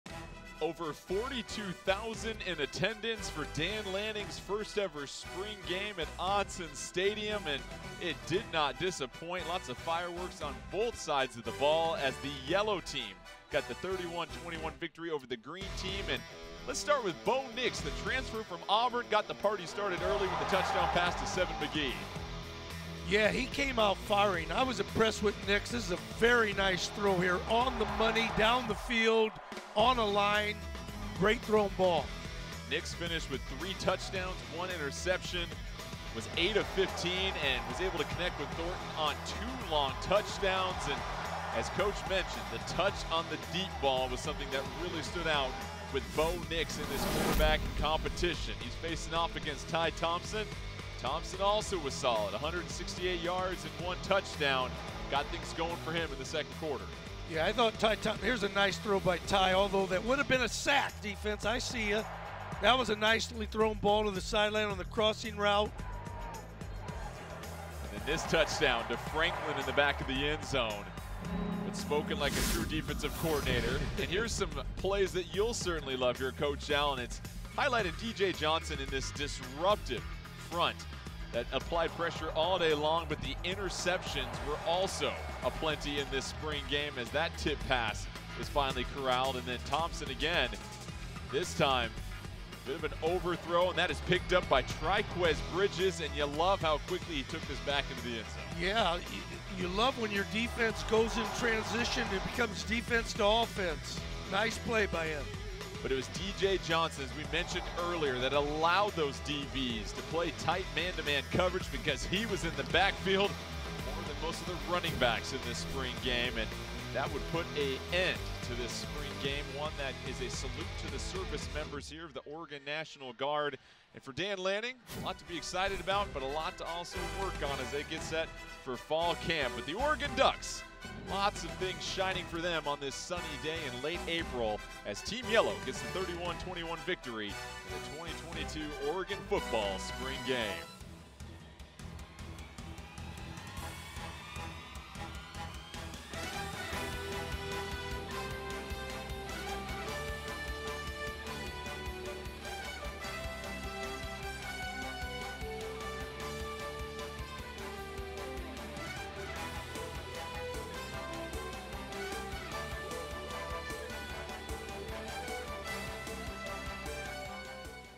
Oregon fans pack Autzen Stadium for Dan Lanning’s first spring game
Pac-12 Networks crew recaps Oregon football's 2022 Spring Game from Eugene.